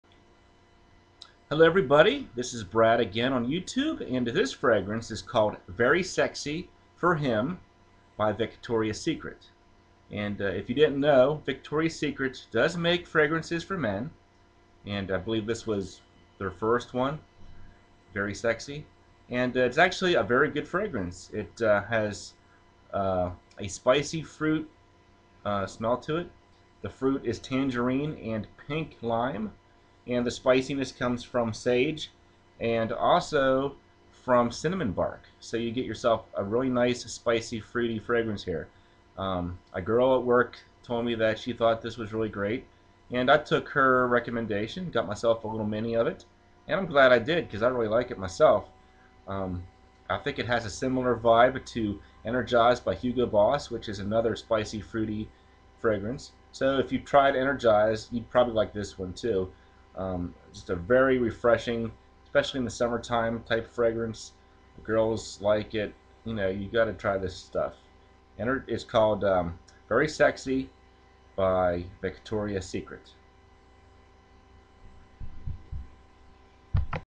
Brad discusses Very Sexy by Victoria Secret
A spicy fruity fragrance.